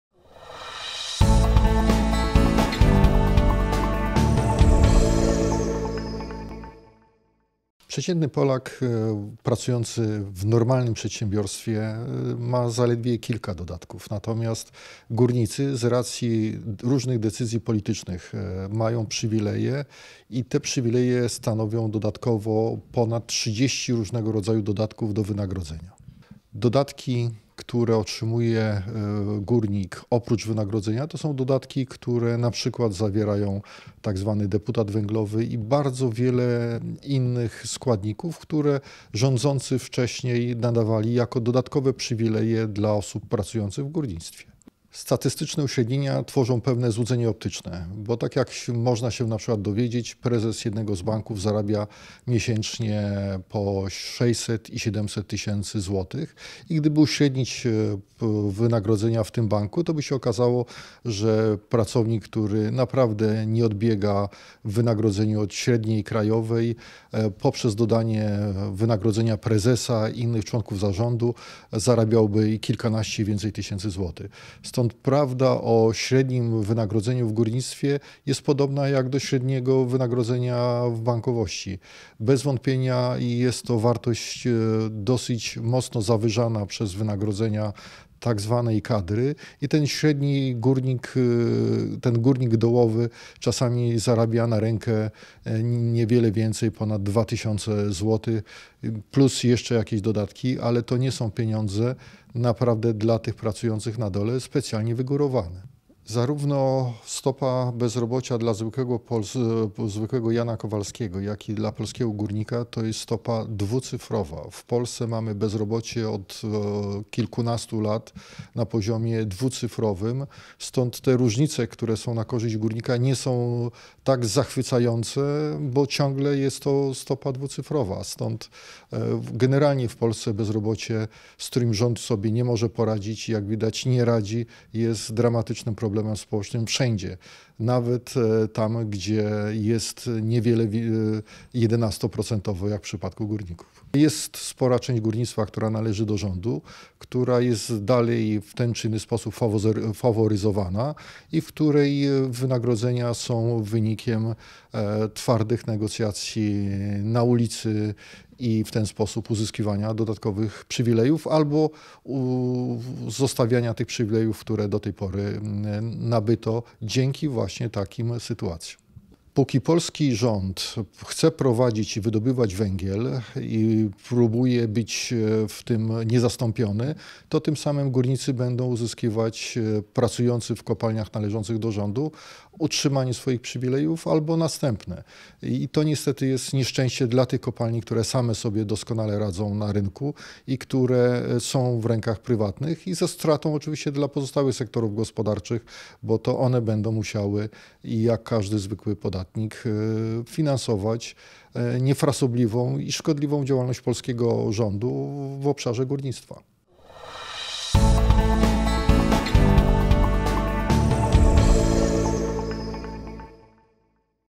Górnik nie takim bogaczem?
- Przeciętny Polak, pracujący w „normalnym” przedsiębiorstwie, ma zaledwie kilka dodatków do wynagrodzenia. Natomiast górnicy mają ich ponad 30. Zawierają one chociażby tzw. deputat węglowy i wiele innych, nadawanych przez rządzących osobom pracującym w górnictwie – mówi Andrzej Sadowski, wiceprezydent Centrum im. Adama Smitha. Jednak jego zdaniem na tych przywilejach znaczące różnice się kończą.